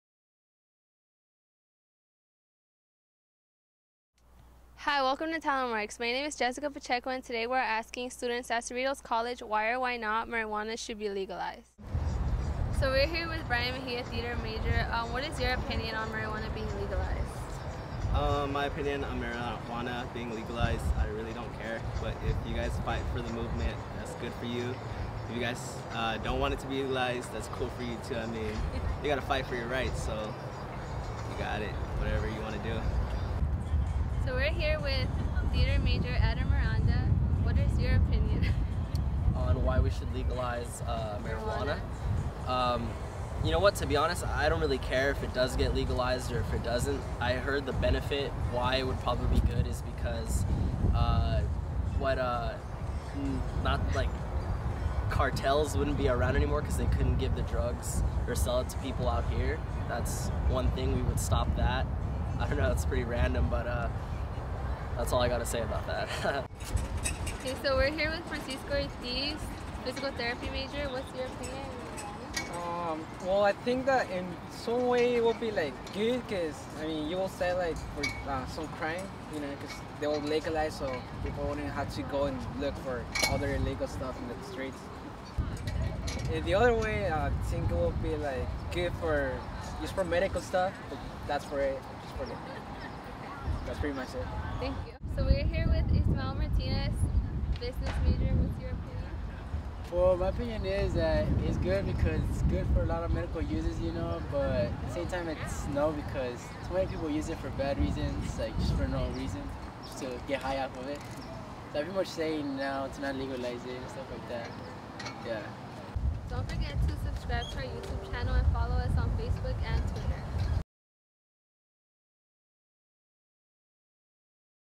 Free speech zone: Should marijuana be legalized?
We talked to several Cerritos College students about the legalization of marijuana.